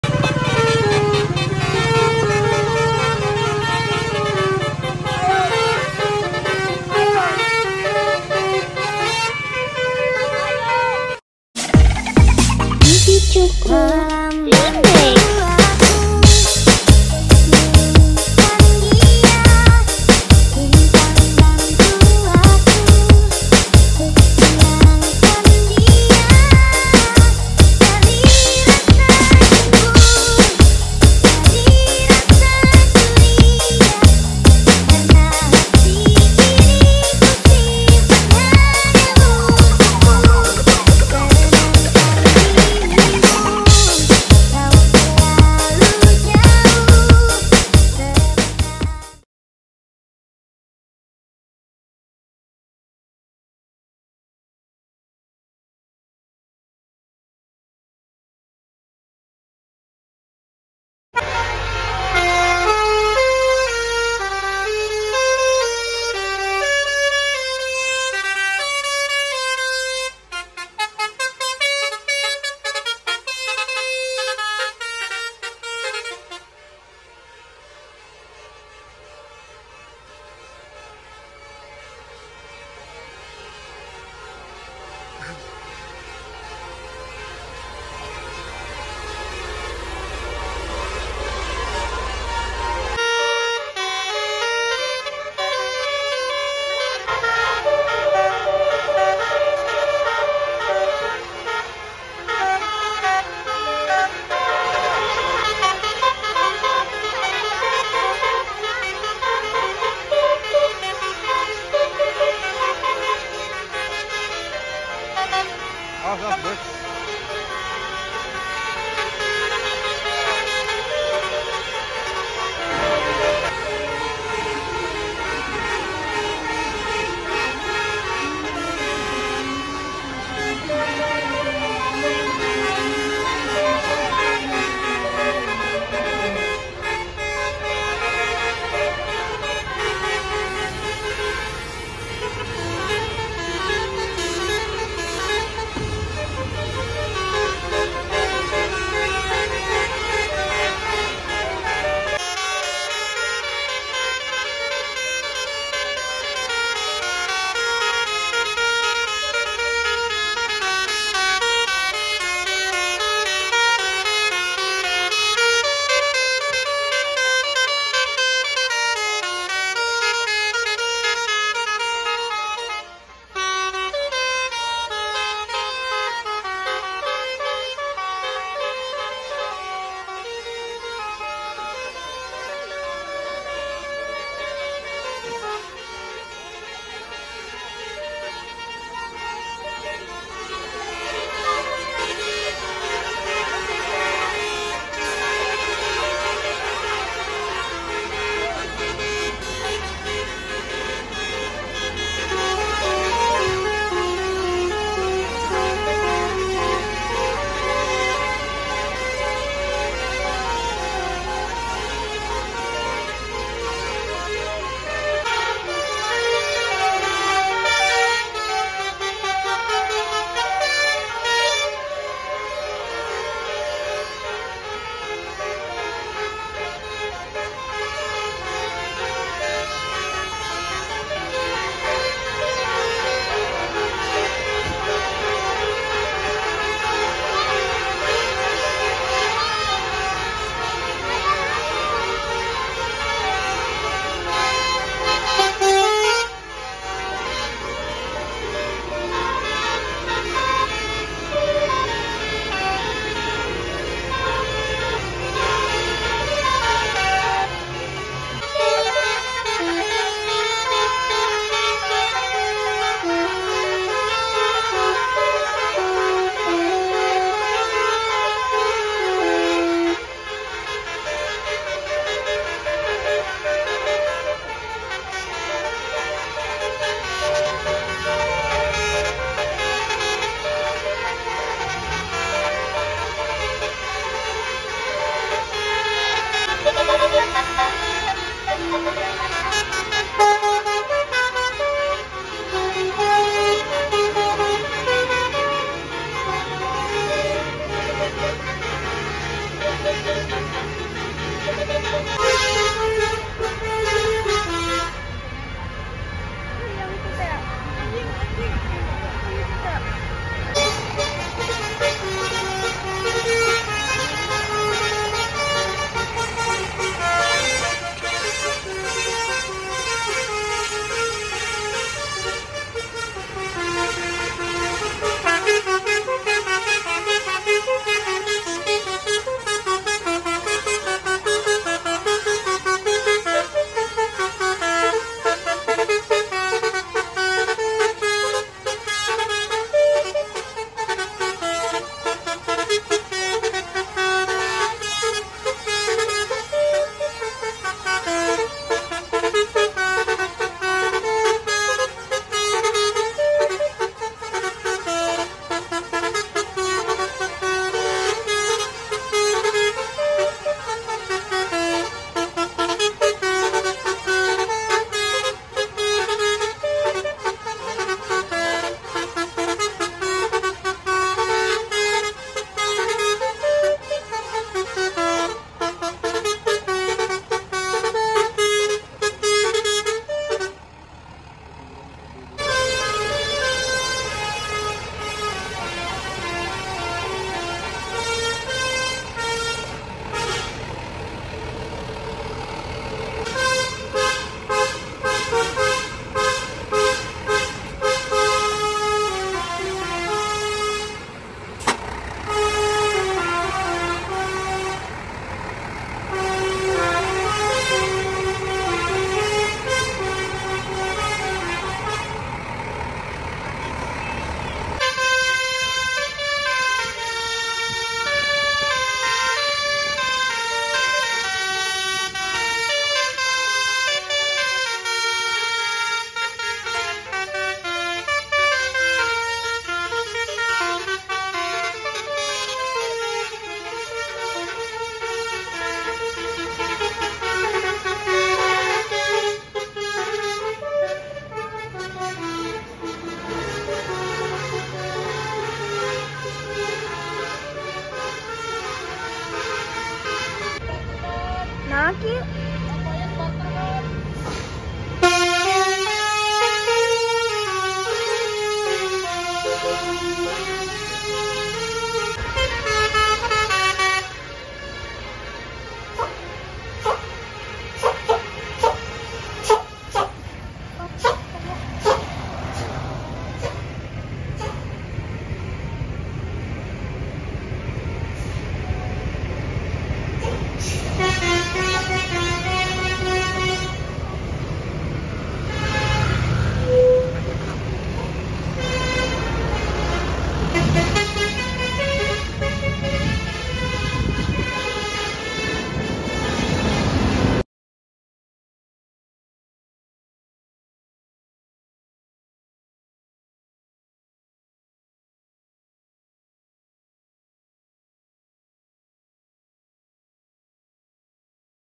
Kumpulan Bis Telolet Paling Seru - Banyak Bis Artis Full Telolet Basuri
Kumpulan bis bis artis full telolet terbaru, terimakasih yang sudah bergabung jangan lupa dukung chanel Midio Jalan ini ya agar kita tambah semangat lagi ngupload bis bis Telolet basuri nya.